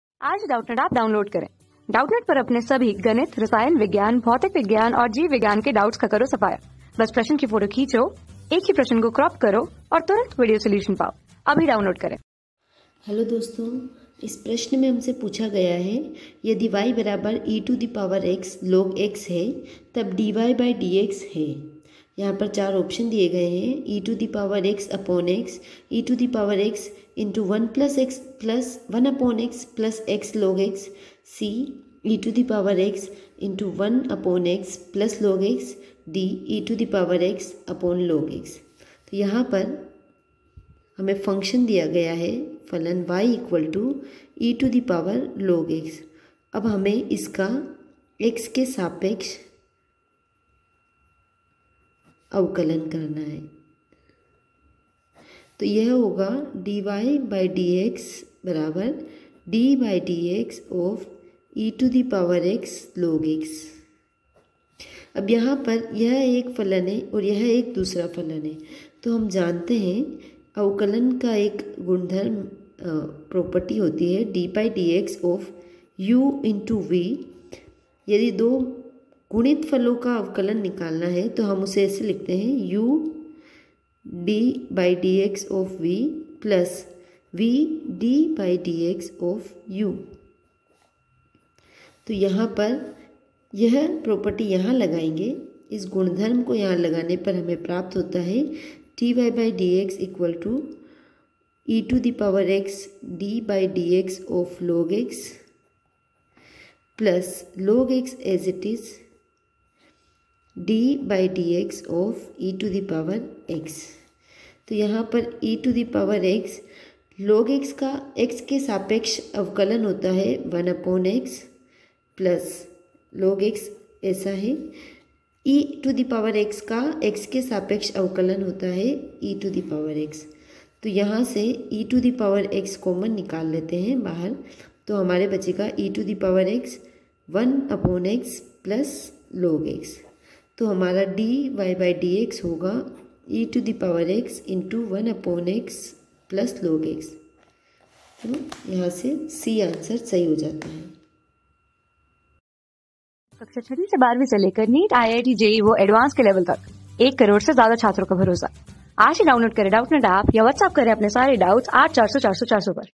यदि y=e^(x)logx, तब (dy/dx) है | 12 | अवकलन तथा अवकलज के अनुप्रयोग | MATHS | ERRORLESS HINDI...
यदि y=e^(x)logx, तब (dy/dx) है
Class: 12
Subject: MATHS
Chapter: अवकलन  तथा  अवकलज  के अनुप्रयोग
Board:NEET

👉 Have Any Query? Ask Us.